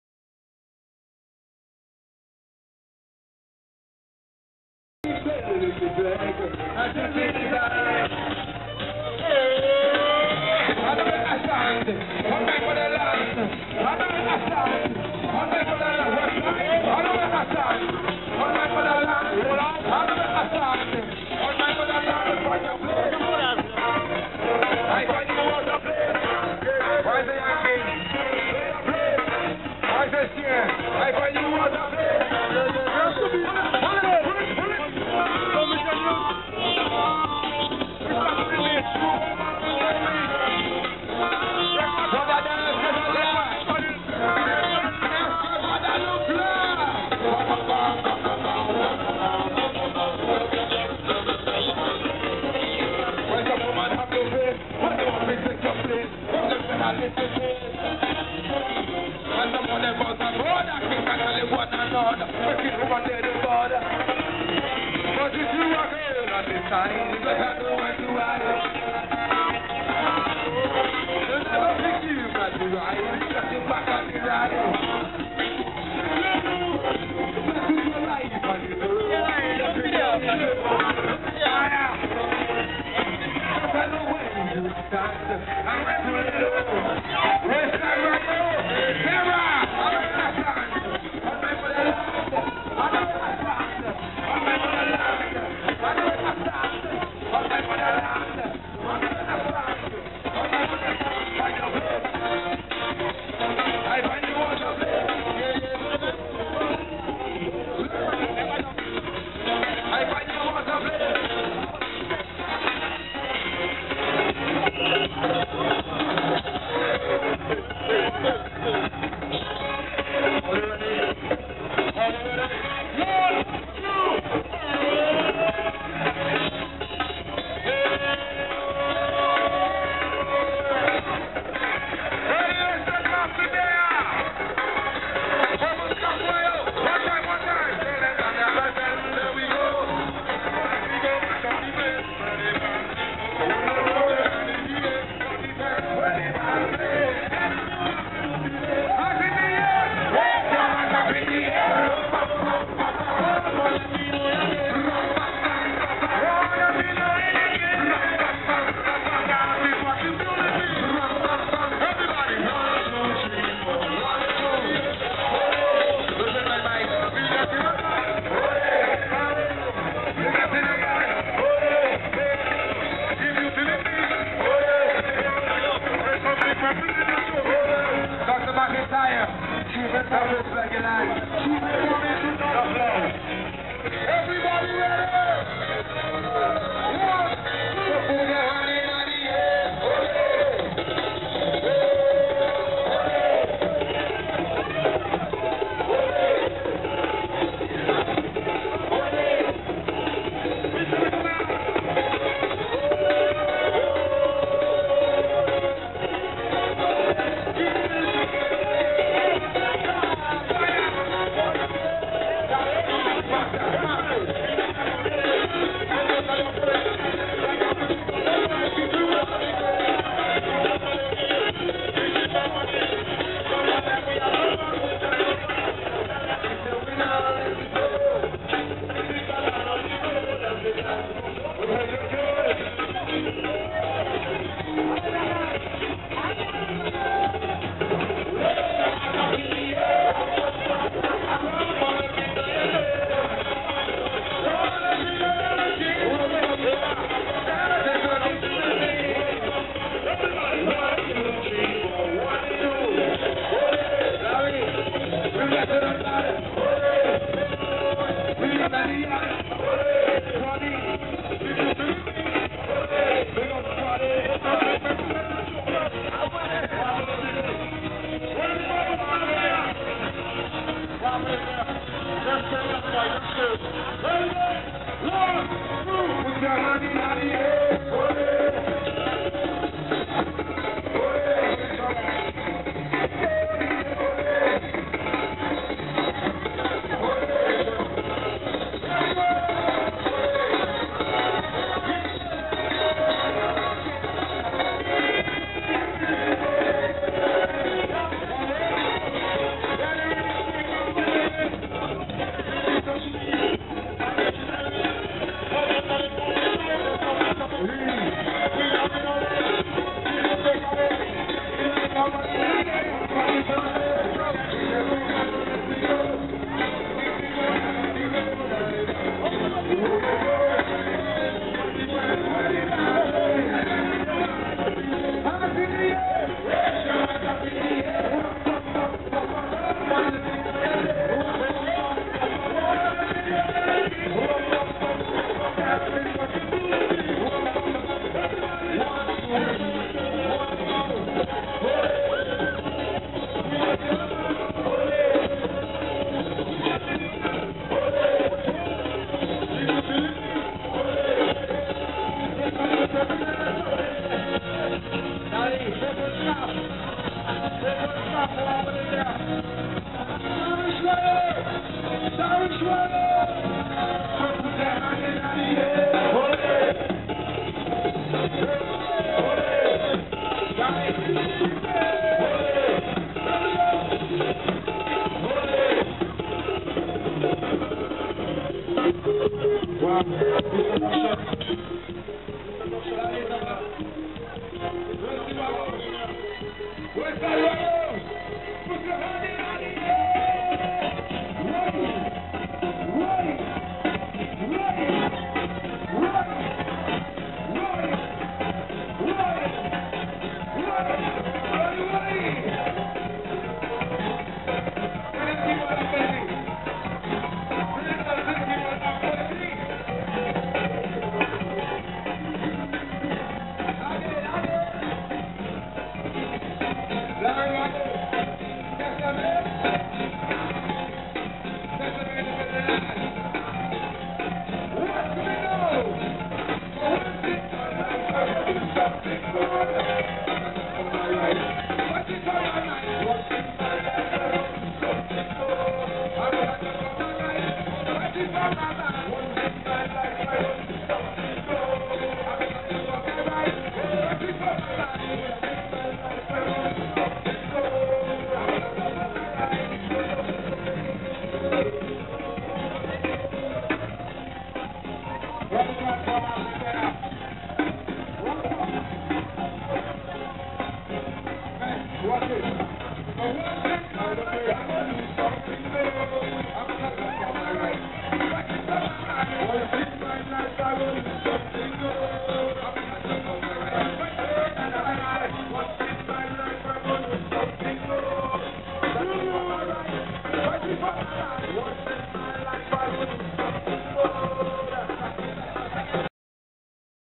WCK live Jou Vert morning action in Roseau
This is WCK playing for Jou Vert morning in Roseau, Dominica for Carnival 2010.